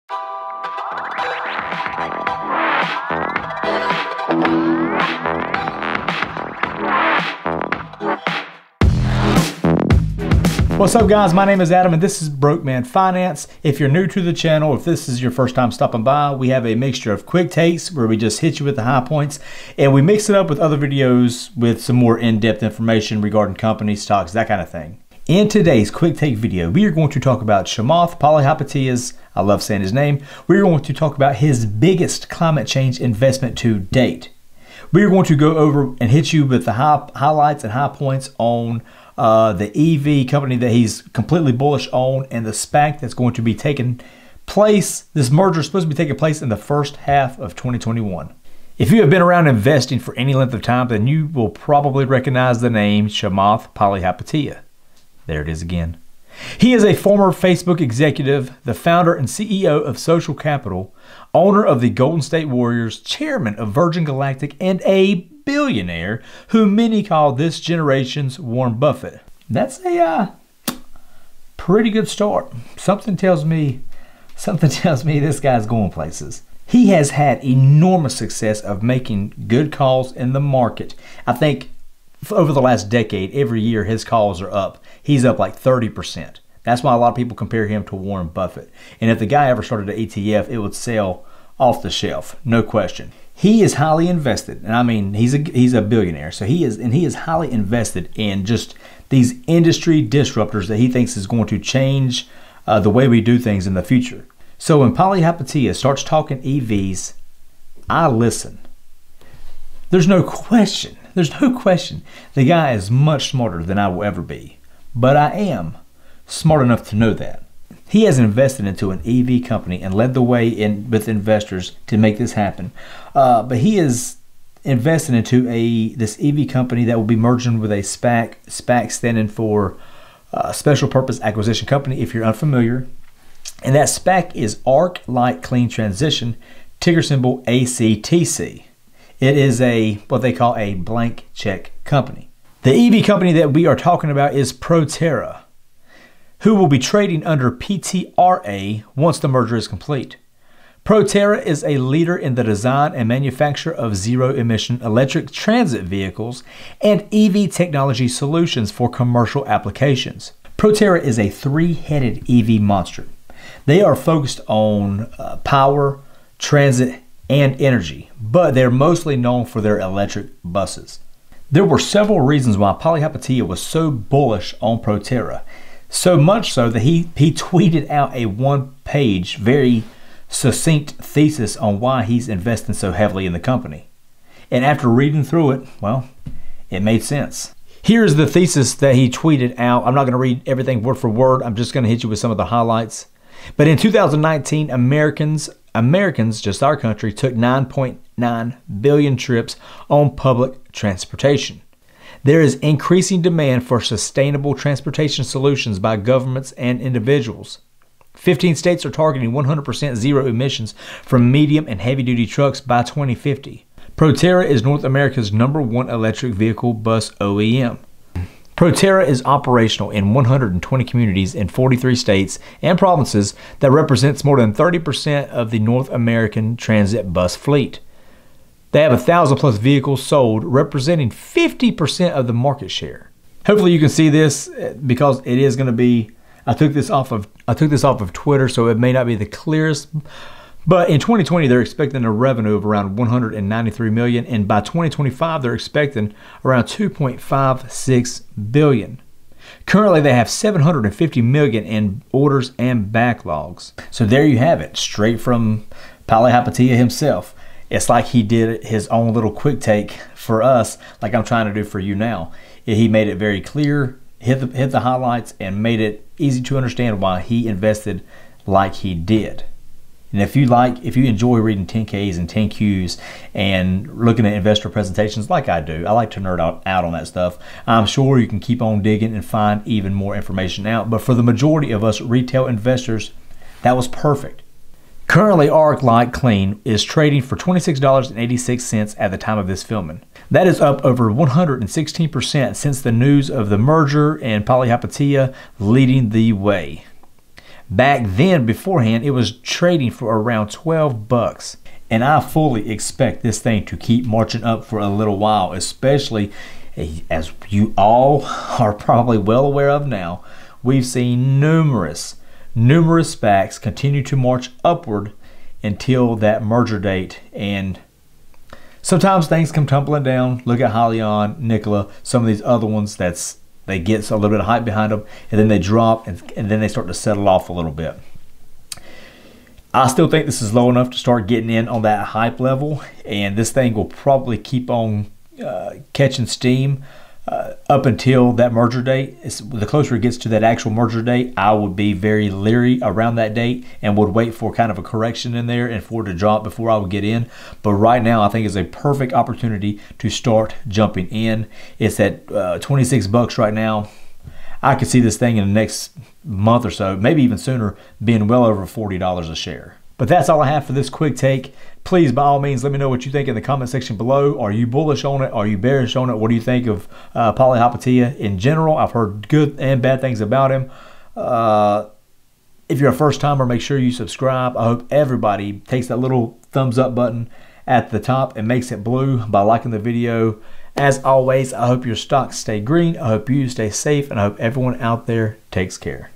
Billionaire Chamath Palihapitiya's Next EV SPAC | ACTC Stock Will Double Soon!
considered as advice. This video was made for entertainment
purposes only.

In today’s quick take video, we are talking about Chamath Paliahapitya’s biggest climate change investment into this EV company.

If you have been around investing for any length of time, then you will probably recognize the name Chamath Paliahaptiya. He is a former Facebook executive, the founder and CEO of Social Capital, owner of the Golden State Warriors, Chairman of Virgin Galactic, and a billionaire, and who many call this generations Warren Buffett.

That’s a good start, something tells me this guy’s going places.

He has had enormous success in making lots of good calls in the market. He is highly invested in what he feels like is future industry disruptors. So, when Chamath starts talking EV’s, I’m listening. Because he’s smarter than I am and I’m smart enough to know that.

Well, he has invested into an EV company that will be merging with a SPAC, or Special Purpose Acquisition Company, ArcLight Clean Transition, a “blank check” company, ticker symbol, ACTC.

This EV company, is Proterra, who will be trading under PTRA once the merger is complete. Proterra is a leader in the design and manufacture of zero-emission electric transit vehicles and EV technology solutions for commercial applications. With industry-leading durability and energy efficiency based on rigorous U.S. independent testing, Proterra products are proudly designed, engineered and manufactured in America, with offices in Silicon Valley, South Carolina, and Los Angeles.

Proterra is a 3 headed EV monster with a focus on power, transit, and energy. But mostly they are known for their electric buses.

The SPAC will provide resources, including $825 million in cash, to take Proterra's portfolio of commercial EV tech to new levels of growth on the open market. The transaction represents an enterprise value of $1.6 billion for Proterra.

There were several reasons why Palihapitiya was so bullish on Proterra and even tweeted out a 1-page succinct thesis. And after reading it, it made sense.

Read the thesis, First.

Currently, Arclight Clean was trading for $26.86 at the time of this filming. That is up over 116% since the news of the merger and Chamath leading the way. Back then it was trading for around $12 bucks.